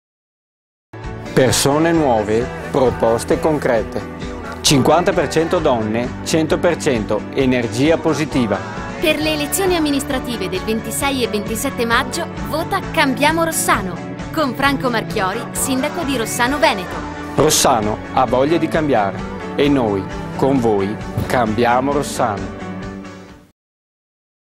cambiamoROSSANO spot tv
spot televisivo delle lista CambiAmo Rossano